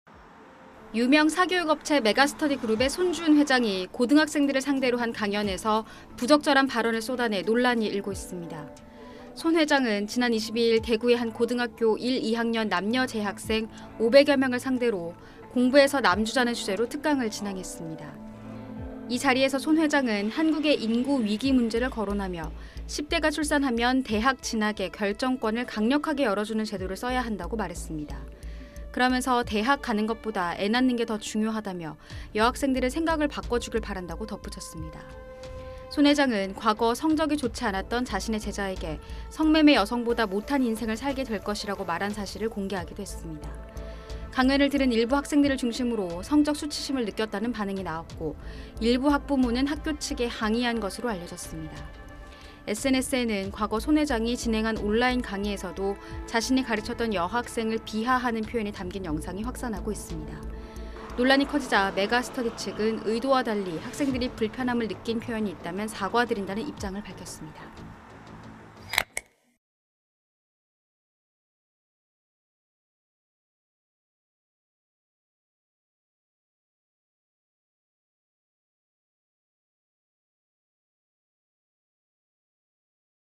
고교 특강서 "공부 안하면 XX보다 못 해" 메가스터디 회장 발언 논란 / SBS / 스브스픽
유명 사교육 업체 메가스터디그룹의 손주은 회장이 고등학생들을 상대로 한 강연에서 부적절한 발언을 쏟아내 논란이 일고 있습니다.

손 회장은 지난 22일 대구의 한 고등학교 1,2학년 남녀 재학생 500여 명을 상대로 '공부해서 남 주자'는 주제로 특강을 진행했습니다.

이 자리에서 손 회장은 한국의 인구 위기 문제를 거론하며 "10대가 출산하면 대학 진학의 결정권을 강력하게 열어주는 제도를 써야 한다"고 말했습니다.

그러면서 "대학 가는 것보다 애 낳는 게 더 중요하다"며 "여학생들은 생각을 바꿔주길 바란다"고 덧붙였습니다.

손 회장은 과거 성적이 좋지 않았던 자신의 제자에게 '성매매 여성보다 못한 인생을 살게 될 것'이라고 말한 사실을 공개하기도 했습니다.

강연을 들은 일부 학생들을 중심으로 "성적 수치심"을 느꼈다는 반응이 나왔고, 일부 학부모는 학교 측에 항의한 것으로 알려졌습니다.

SNS에는 과거 손 회장이 진행한 온라인 강의에서도 자신이 가르쳤던 여학생을 비하하는 표현이 담긴 영상이 확산하고 있습니다.

논란이 커지자 메가스터디 측은 "의도와 달리 학생들이 불편함을 느낀 표현이 있다면 사과드린다"는 입장을 밝혔습니다.

(구성 : 제희원 / 영상편집 : 김수영 / 화면출처 : X / 제작 : 디지털뉴스편집부)